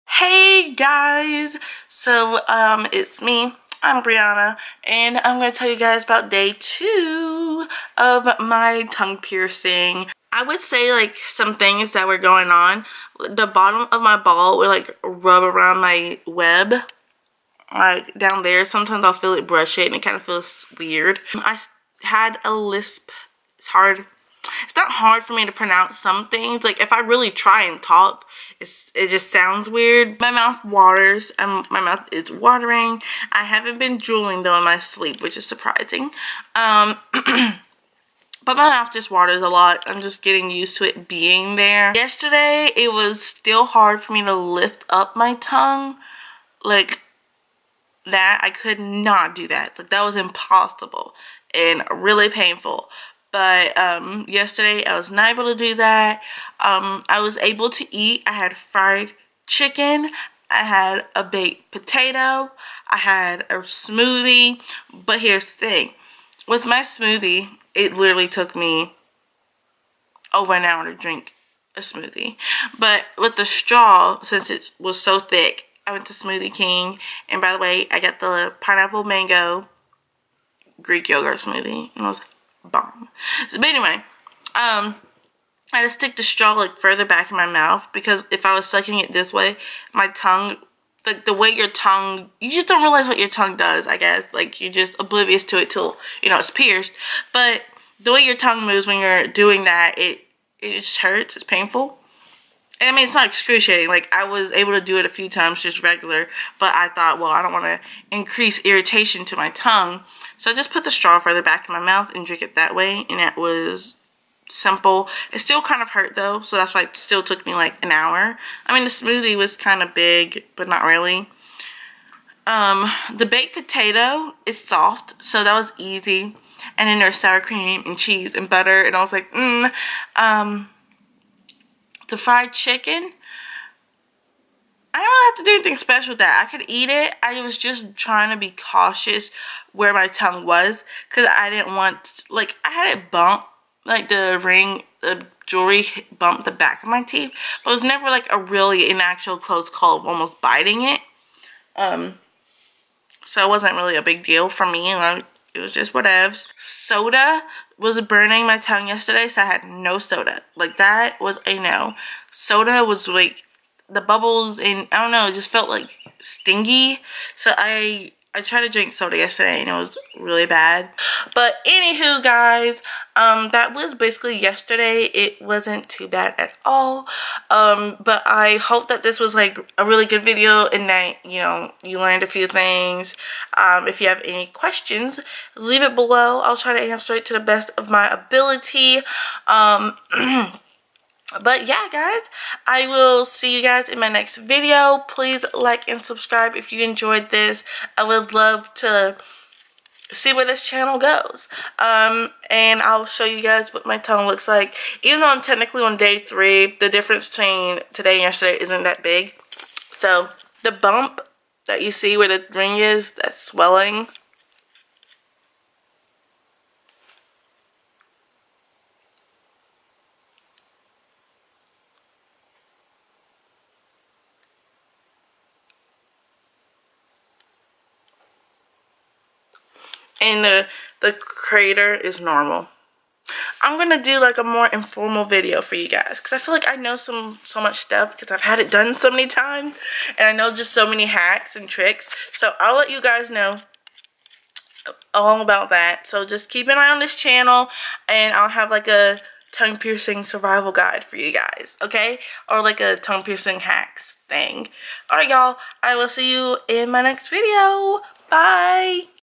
Tongue Piercing: Day 2
Here is all the craziness from day 2 of m tongue piercing. Luckily each day my tongue piercing is easier to manage. So feel free to watch and let me know down below what you experienced on day 2 of your tongue piercing.